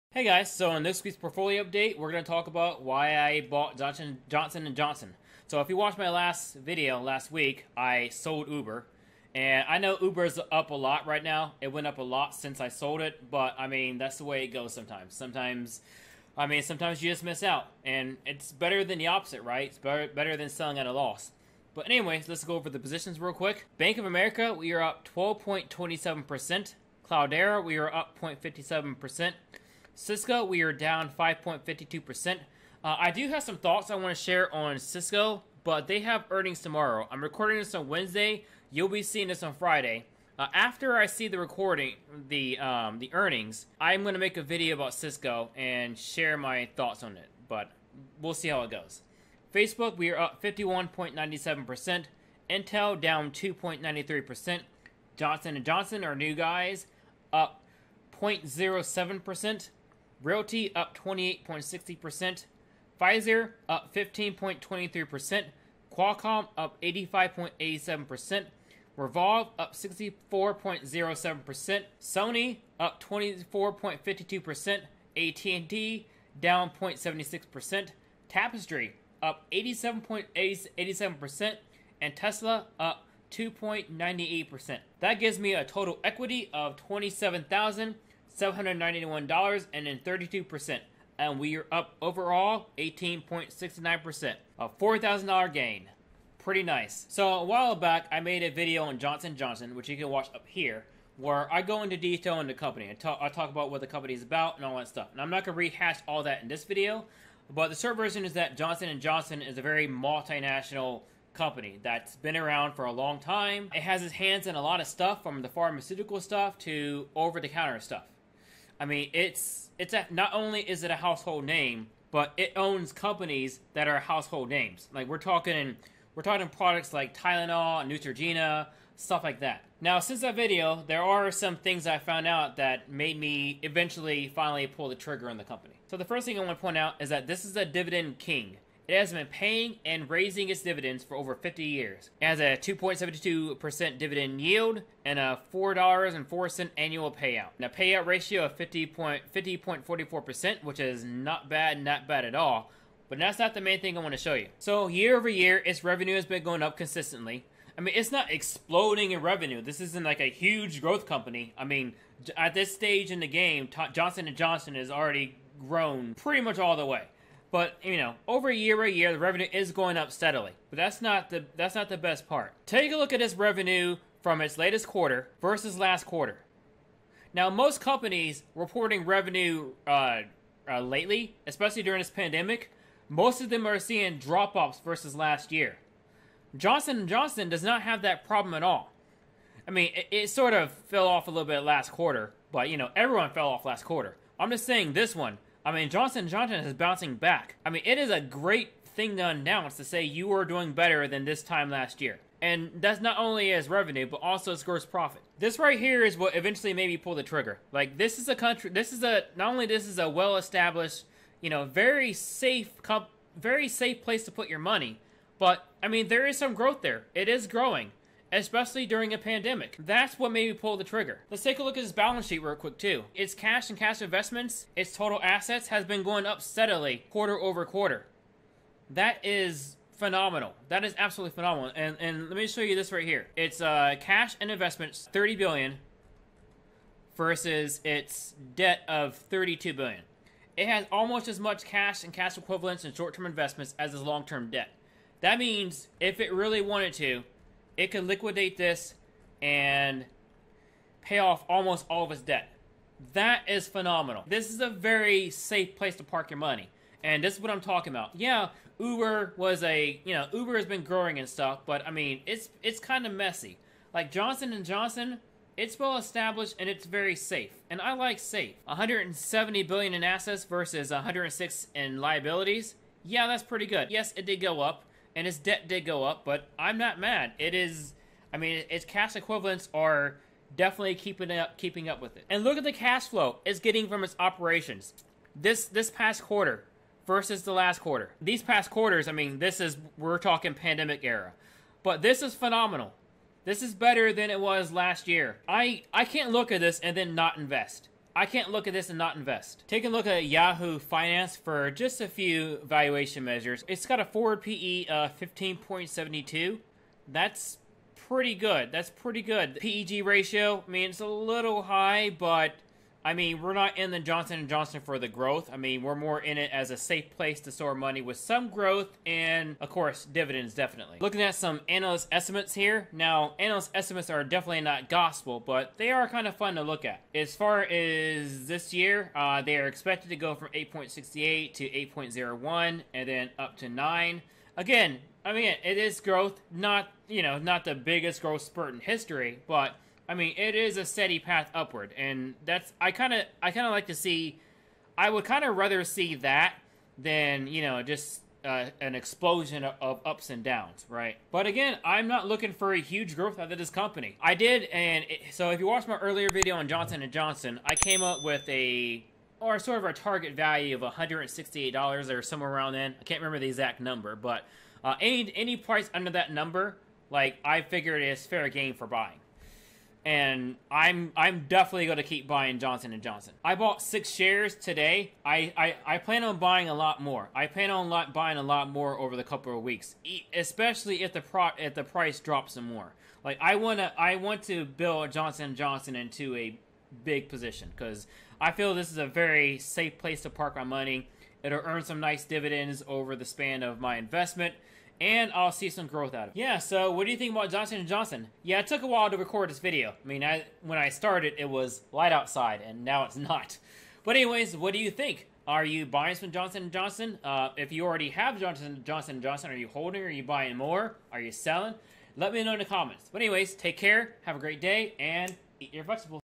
Why I bought Johnson & Johnson Stock | Stock Portfolio Update (Part 22)
I finally bought Johnson and Johnson stock!

This was a company I've been slowly getting into over the past few months. This week I decided to finally pull the trigger and pick up some shares. The goal is to deploy around $2500 into the stock, and then increment it bit by bit as the months roll on.

Also, we'll go over my Fidelity Investments portfolio just like we do every week. You'll see every stock and its performance for the life of the portfolio.